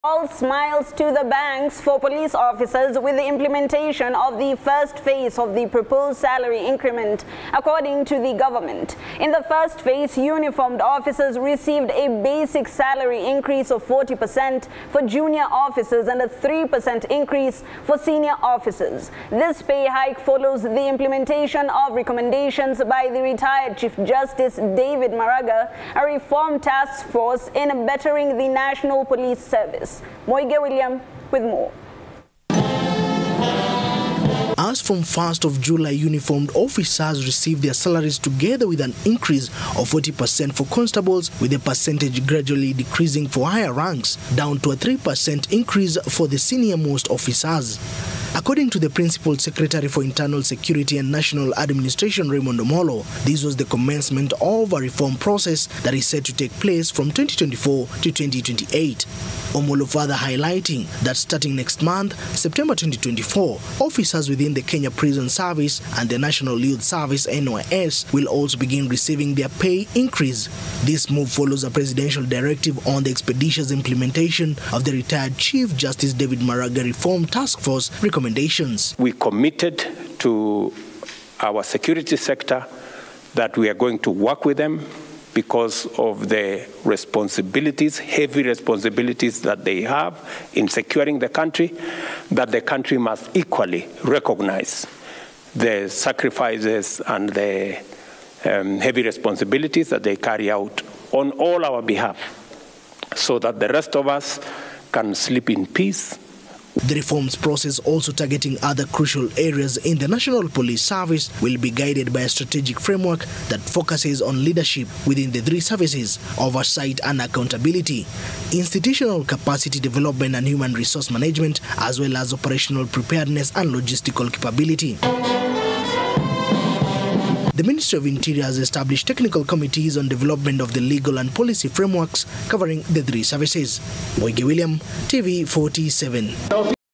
Police officers receive salary increase
It is all smiles to the banks for police officers with the implementation of the first phase of the proposed salary increment.